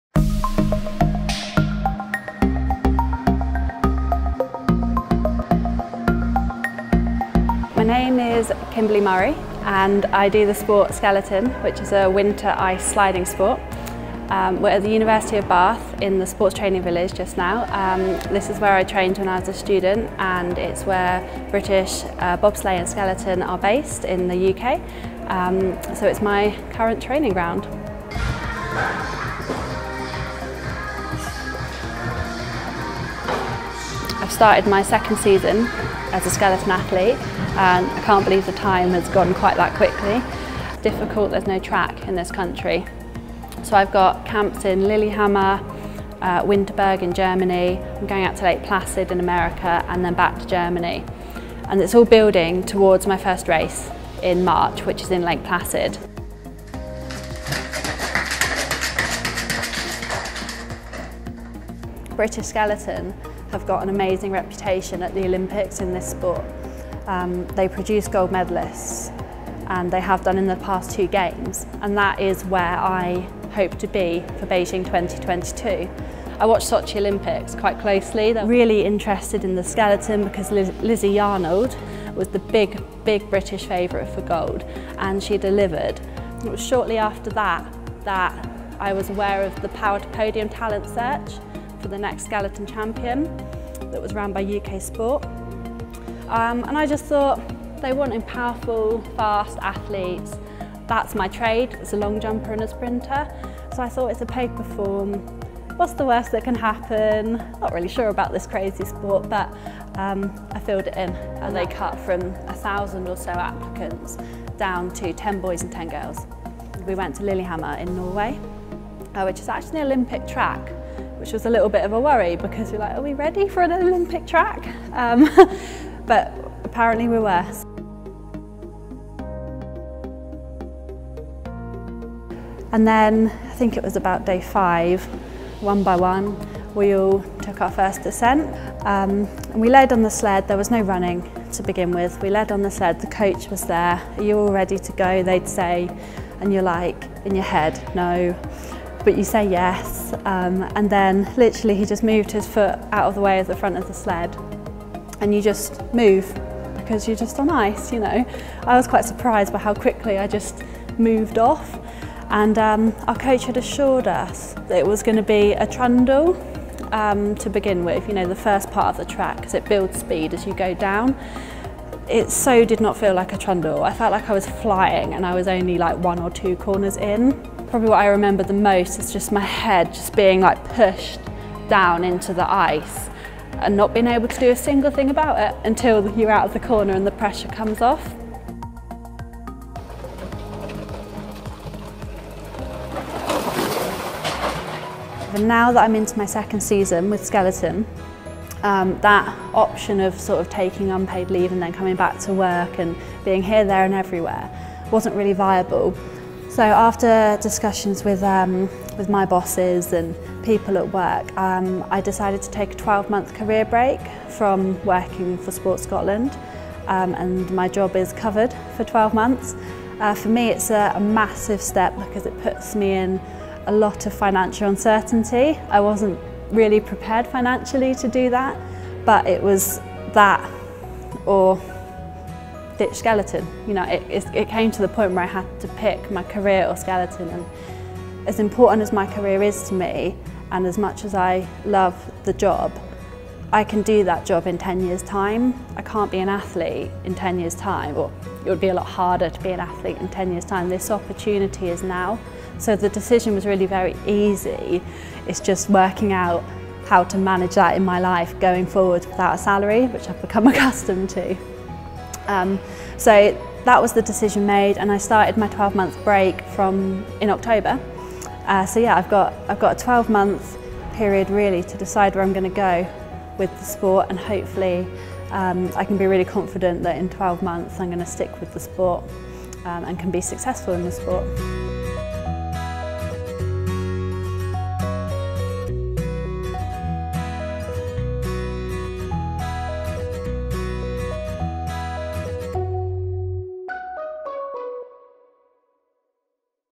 Kimberley Murray - GB Skeleton Athlete
Kimberly Murray is attempting to make it to the 2022 Winter Olympics in Beijing. Her plan is to compete in The Skeleton and become part of a great Team GB tradition from Amy Williams, through to Lizzie Yarnold and Laura Deas. This is the beginning of her story.